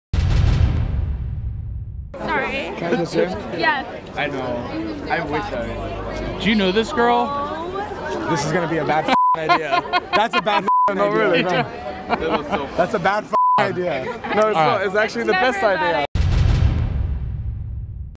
Vanderpump Rules' Star James Kennedy -- Watch Me Suck Random Face | TMZ
James Kennedy from "Vanderpump Rules" is a real charmer ... and he shows us his mac powers in full glory.